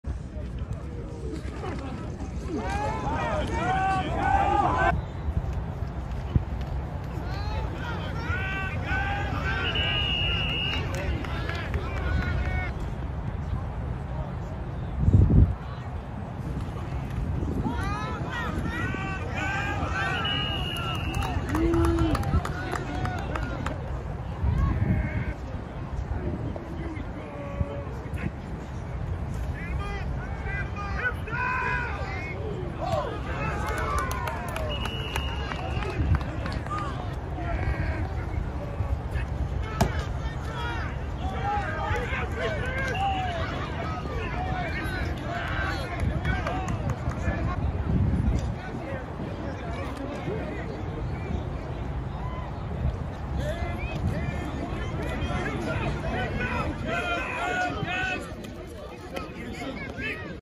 Super Tiny RB Deuce Vaughn quickly hits the gap shows off top speed
Super Tiny Dallas Cowboys RB Deuce Vaughn quickly disappears in traffic as he hits the gap showing off top speed.
Ezekiel Elliott, Rico Dowdle, Deuce Vaughn and Royce Freeman in a heated running back competition.